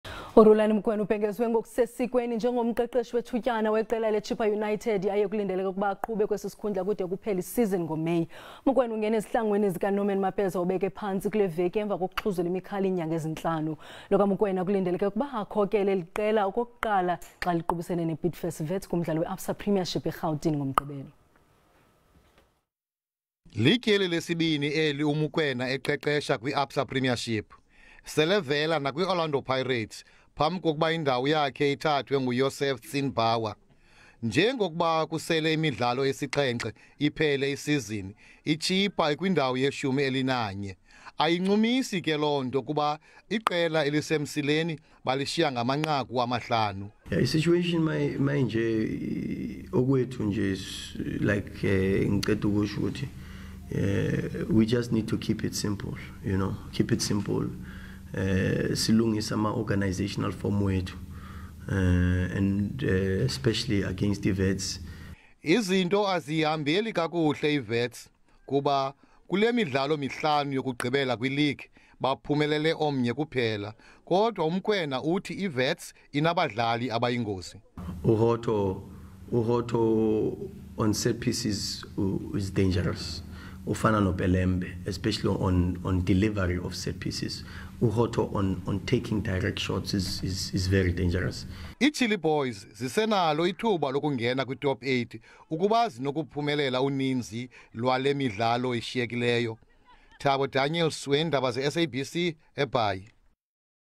Ngumqeqeshi omtsha weChippa uRhulani Mokoena
uRhulani Mokoena ubhengezwe ngokusesikweni njengomqeqeshi wethutyana weqela le Chippa United yaye kulindeleke ukuba aqhube kwesisikhundla kude kuphele iSeason ngo May. UMokoena ungena ezihlangwini zika Norman Mapeza obeke phantsi kuleveki emva kokuxhuzula imikhala iinyanga ezintlanu. Lo ka Mokoena kulindeleke ukuba akhokele eliqela okokuqala xaliqubisene ne Bidvest Wits kumdlalo we-ABSA Premiership eRhawutini ngomgqibelo.

For more news, visit sabcnews.com and also SABCNews SABCIindaba Ezemidlalo on Social Media.